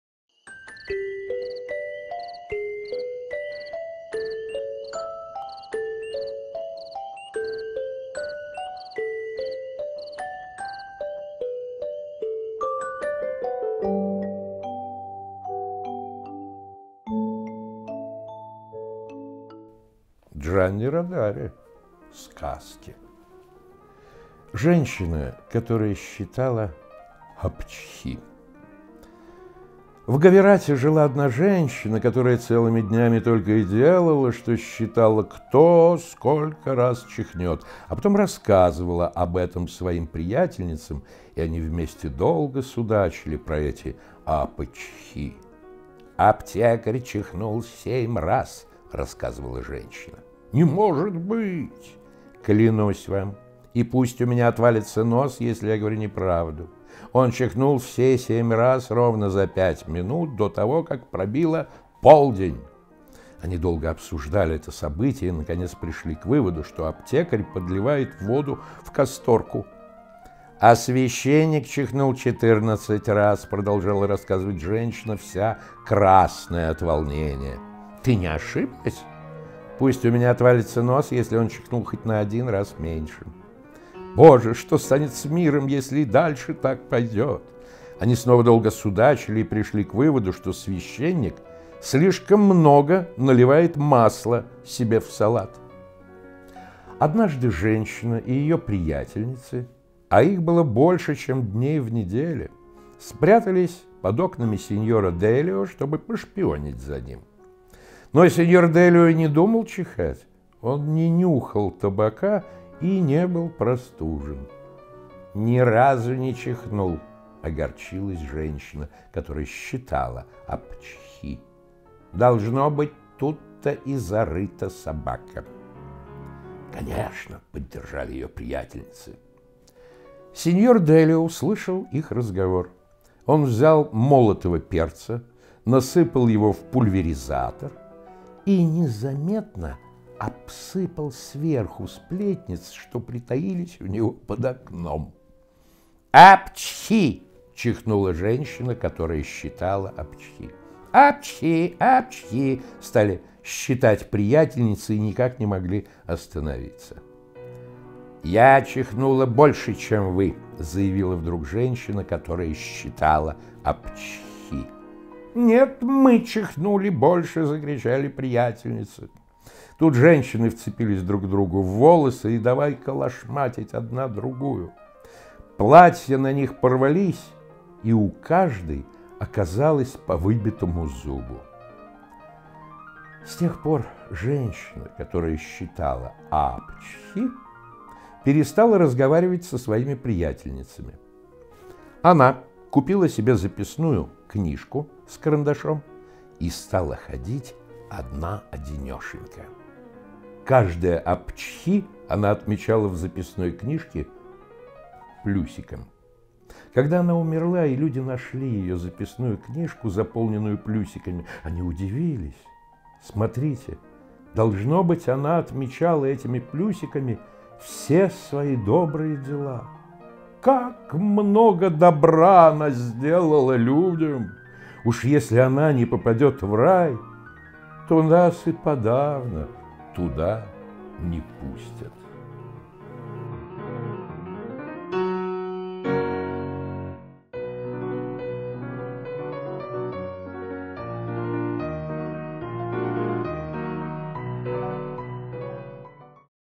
Николай Буров читает сказку Джанни Родари "Женщина, которая считала апчхи"
Николай Буров - советский, российский актёр и общественный деятель, директор Санкт-Петербургского государственного бюджетного учреждения культуры "Государственный музей-памятник "Исаакиевский собор" в 2008—2017 годах. Народный артист Российской Федерации (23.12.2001).